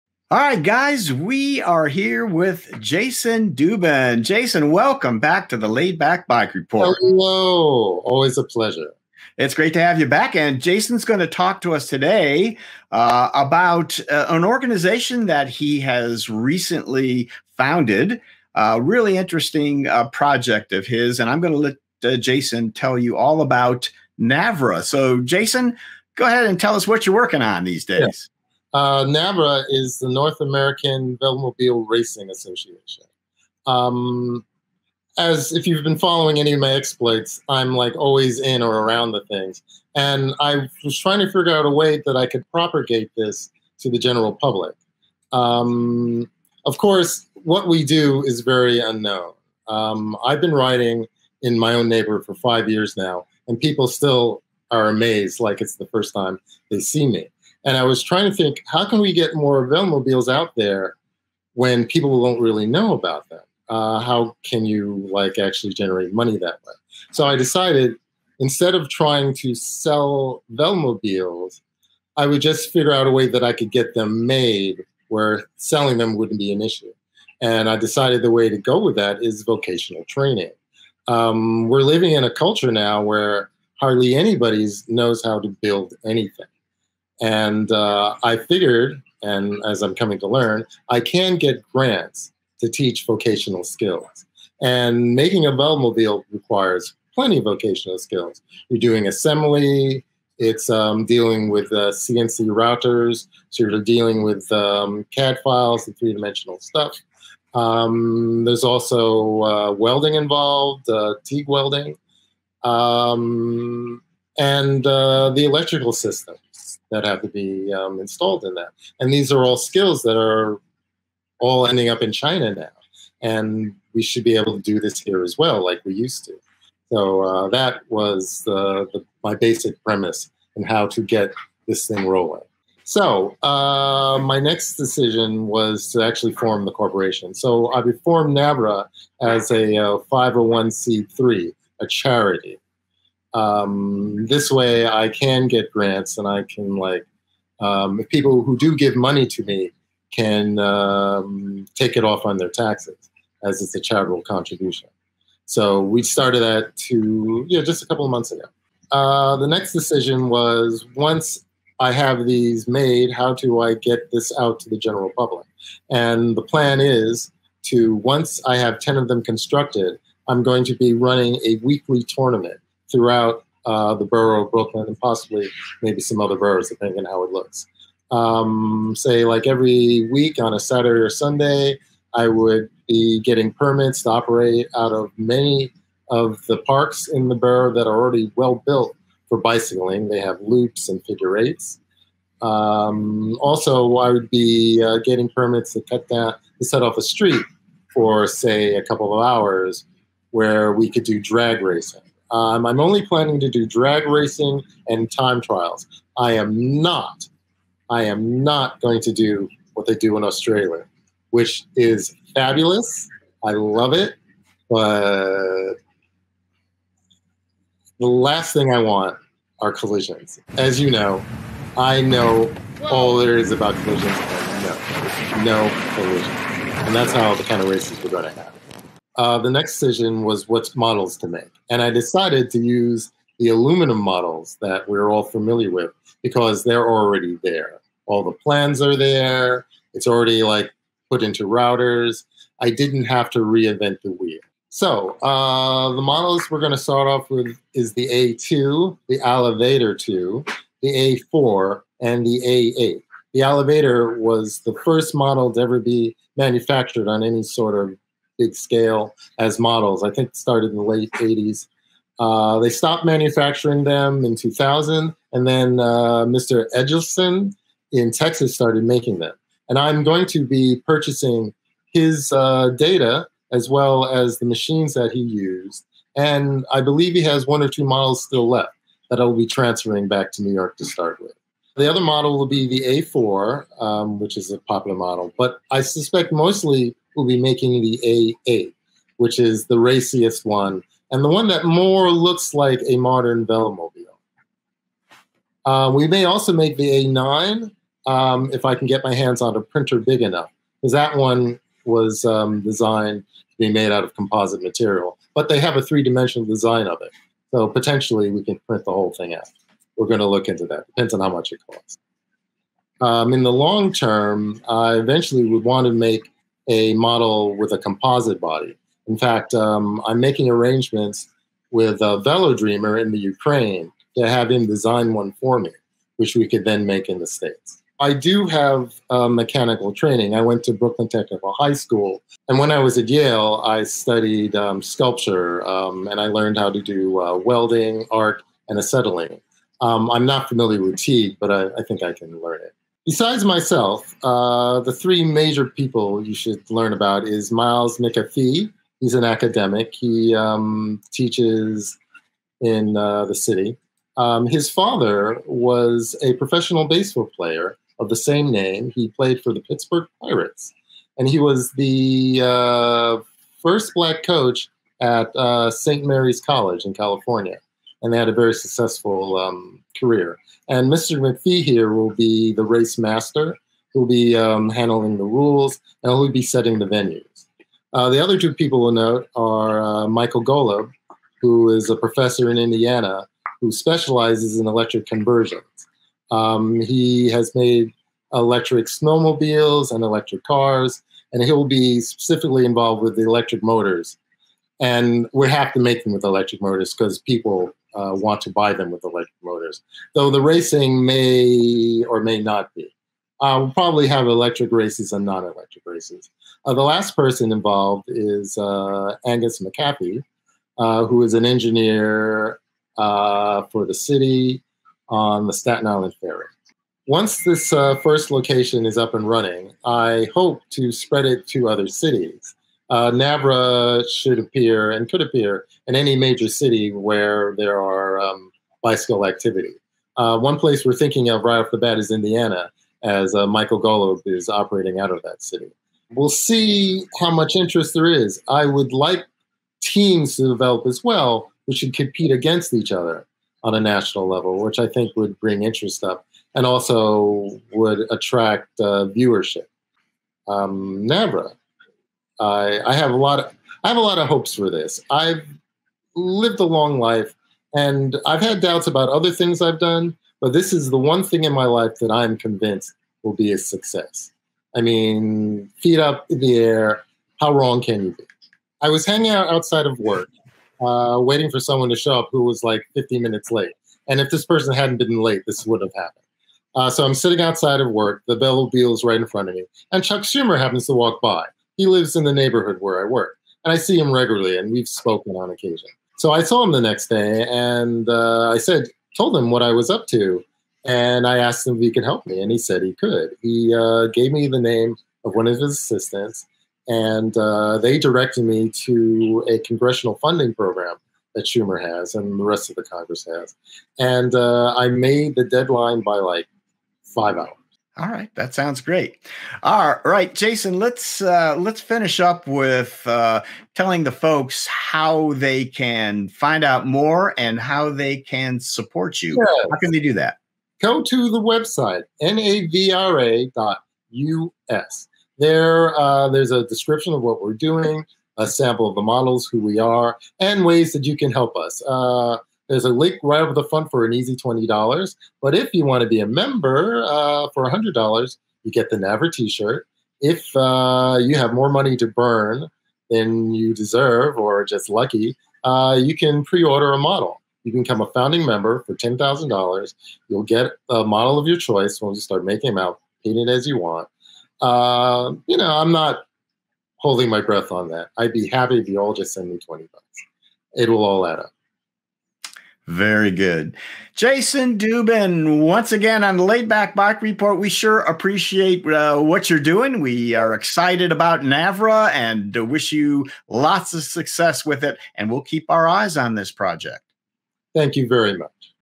Building/Racing Velomobiles with NYC Youth-NAVRA
Jason Dubin lives in Brooklyn, New York and hopes to engage the local youth in constructing and then racing velomobiles. He has started by creating an organization named NAVRA or North American Velomobile Racing Association. Listen as Jason describes his background and his ambitious plan to educate and train young people in the mechanical and life skills necessary to survive in today's world.